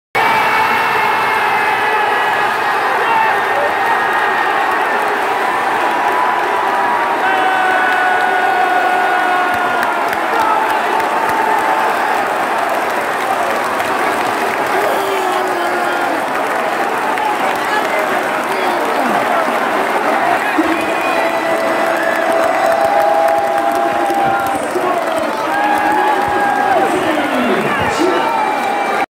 JOSH COBURN SCORES BRISTOL ROVERS FIRST GOAL AGAINST FLEETWOOD TOWN.
JOSH COBURN SCORES BRISTOL ROVERS FIRST GOAL AGAINST FLEETWOOD TOWN TO MAKE IT 1-1. ROVERS THEN TOOK THE LEAD WITH A GOAL FROM AARON COLLINS BUT CONCEDED A GOAL IN THE DYING SECONDS OF INJURY TIME TO END WITH A 2-2 DRAW.